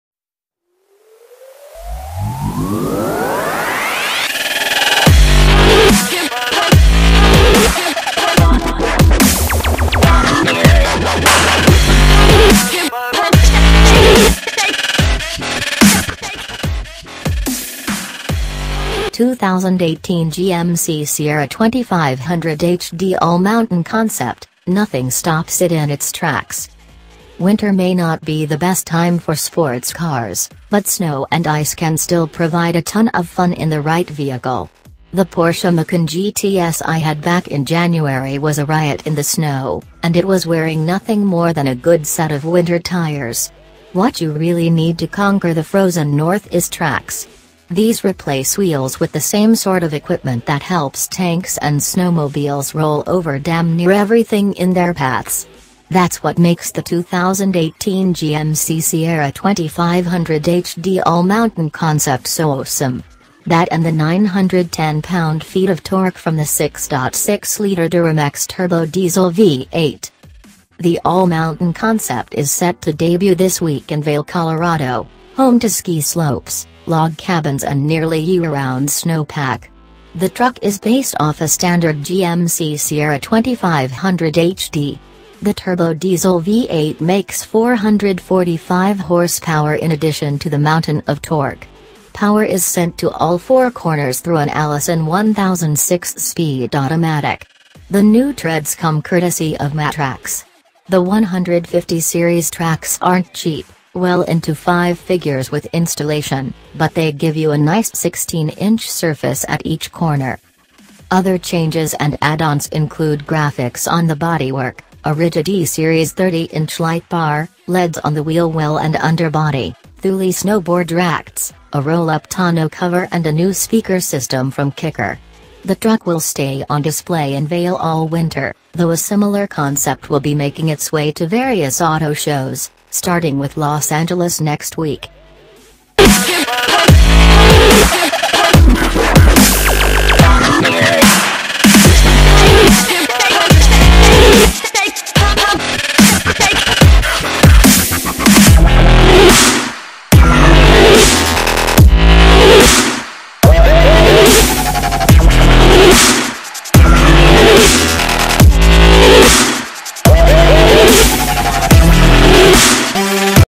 [HOT NEWS] 2018 GMC Sierra 2500HD All Mountain concept
2018 GMC Sierra 2500HD All Mountain concept: Nothing stops it in its tracks
Winter may not be the best time for sports cars, but snow and ice can still provide a ton of fun in the right vehicle. The Porsche Macan GTS I had back in January was a riot in the snow, and it was wearing nothing more than a good set of winter tires. What you really need to conquer the frozen north is tracks. These replace wheels with the same sort of equipment that helps tanks and snowmobiles roll over damn near everything in their paths. That's what makes the 2018 GMC Sierra 2500HD All Mountain concept so awesome. That and the 910 pound-feet of torque from the 6.6-liter Duramax turbo diesel V8.

The All Mountain concept is set to debut this week in Veil, Colorado, home to ski slopes, log cabins and nearly year-round snow pack. The truck is based off a standard GMC Sierra 2500HD. The turbo-diesel V8 makes 445 horsepower in addition to the mountain of torque. Power is sent to all four corners through an Allison 1000 six-speed automatic. The new treads come courtesy of Mattracks. The 150 Series tracks aren't cheap — well into five figures with installation — but they give you a nice 16-inch surface at each corner.